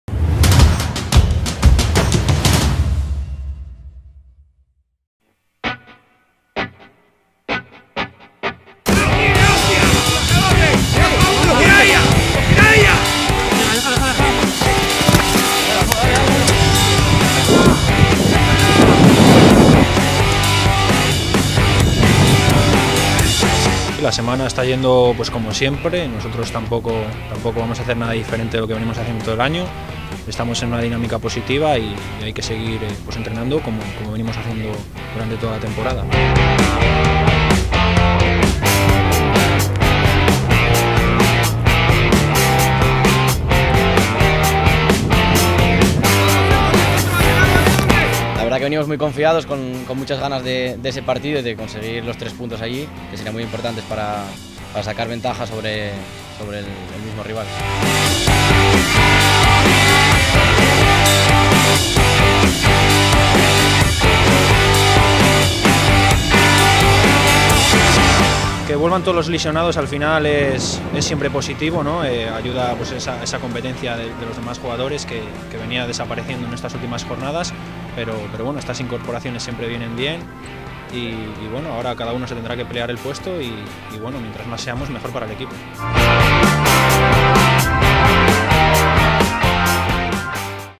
Real Sporting de Gijón (26/1/2018)
Alberto Cayarga y Claudio Medina hacen un repaso a la semana del Sporting B